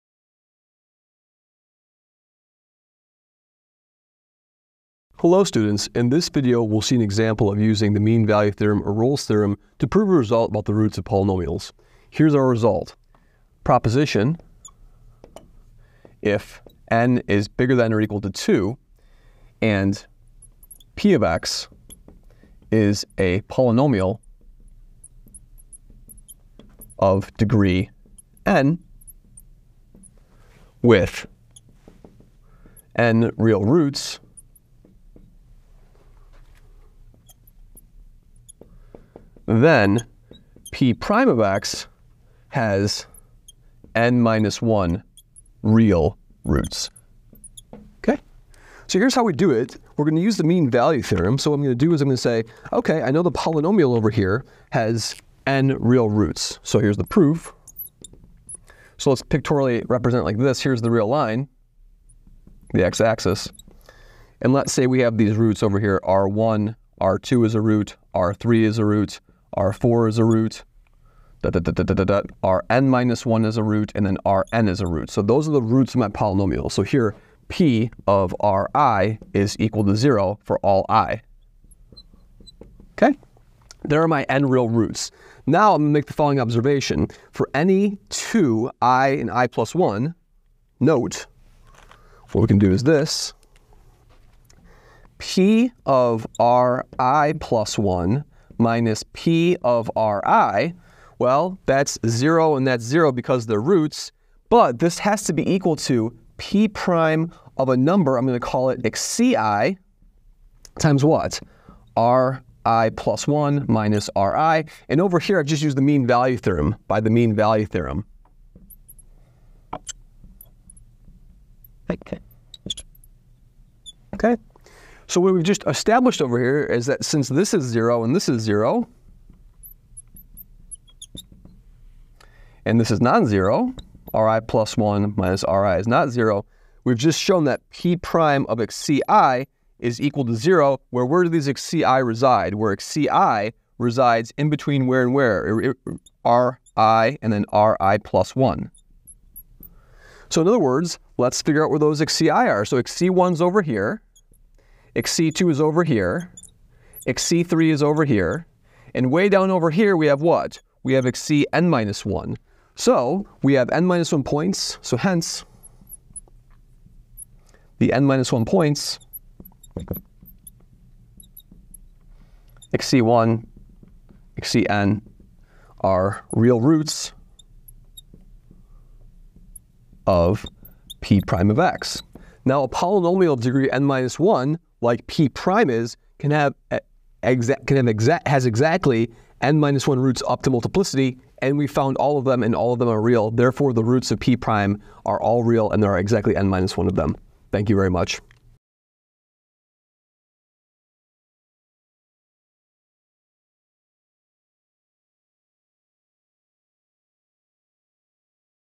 The Derivative of a Polynomial with Only Real Roots Itself Only Has Real Roots
We use the Mean Value Theorem to prove that between two distinct real roots of a polynomial, the derivative of the polynomial must also have a root.